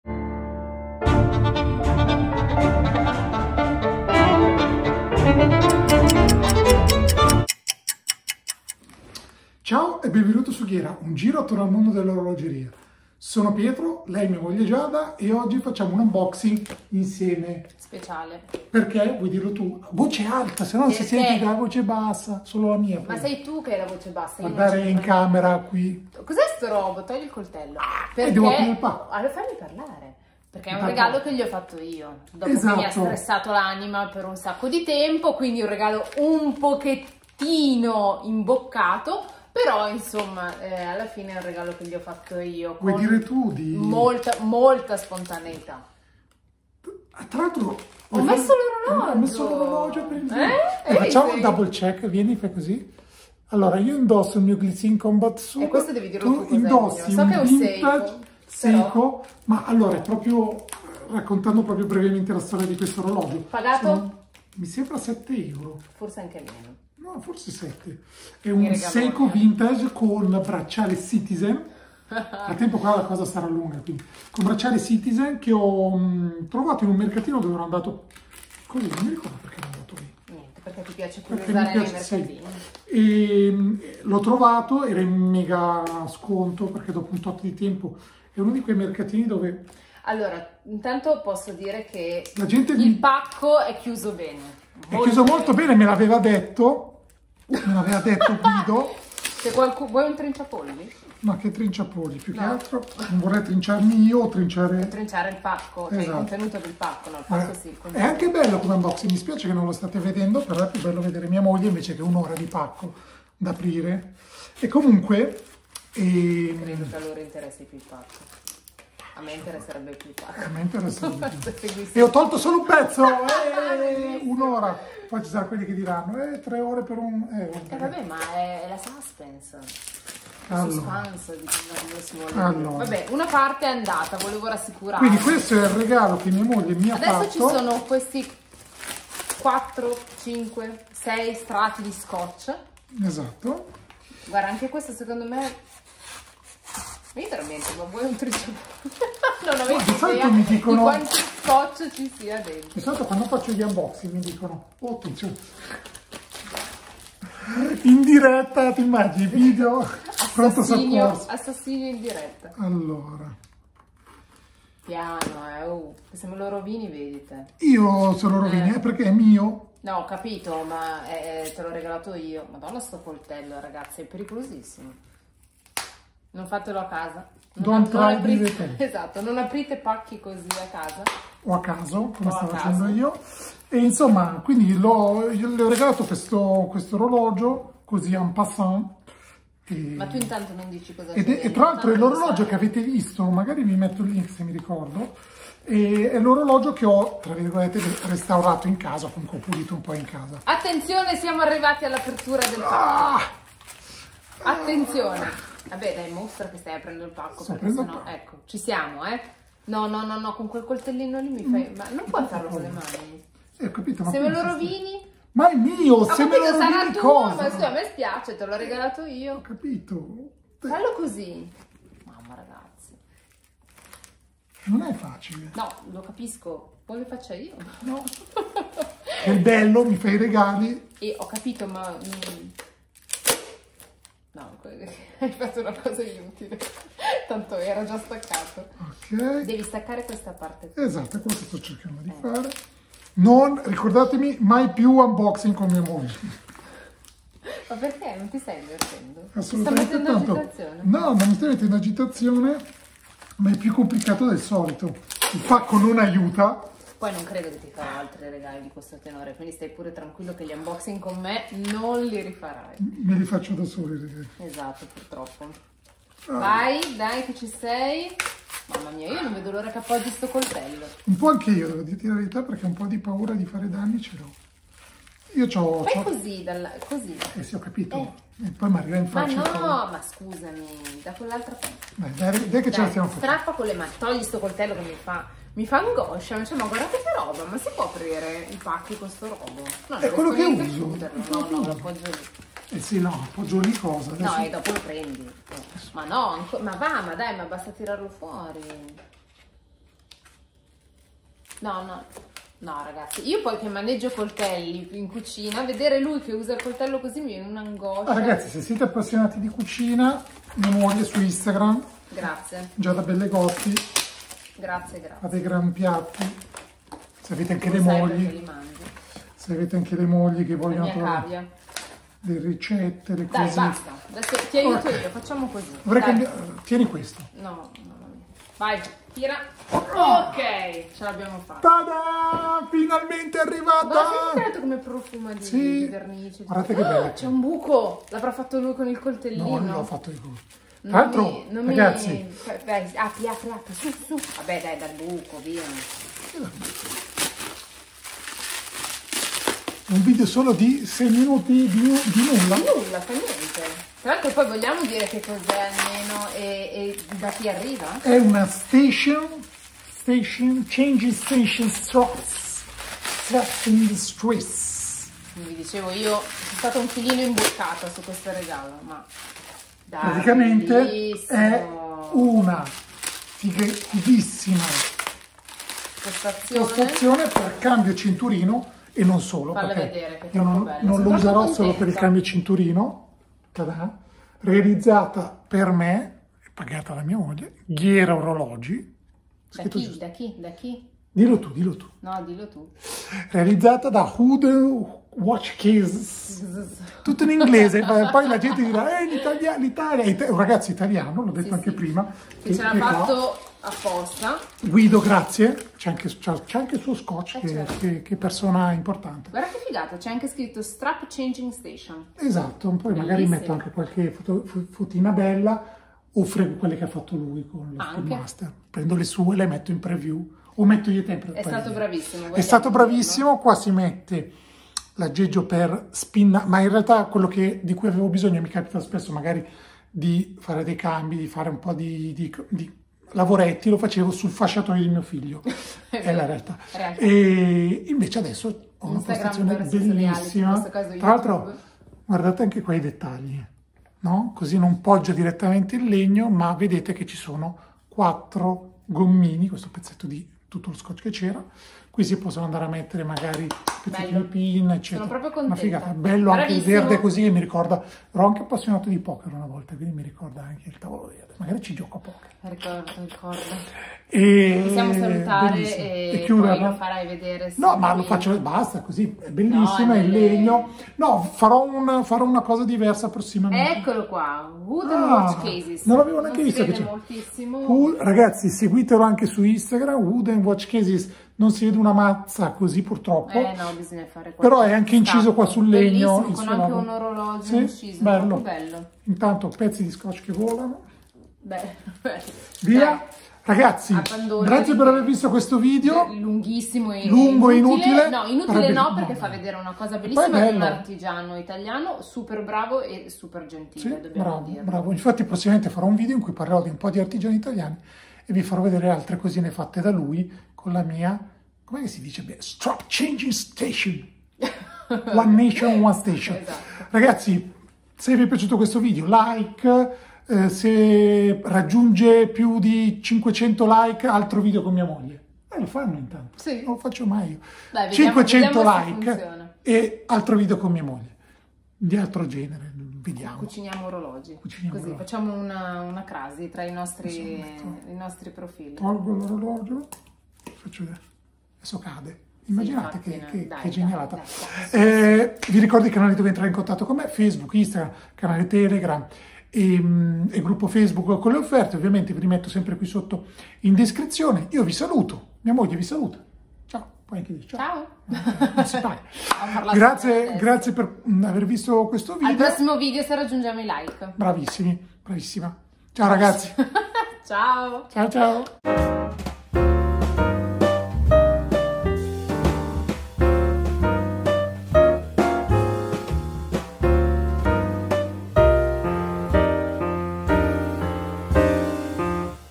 Unboxing in due! Apro il regalo di mia moglie con lei 🎀 (e voi)
Se vuoi sostenere il canale quando acquisti orologi su Amazon passa invece da qui

Se sei appassionato di orologi e sempre alla ricerca dell'offerta migliore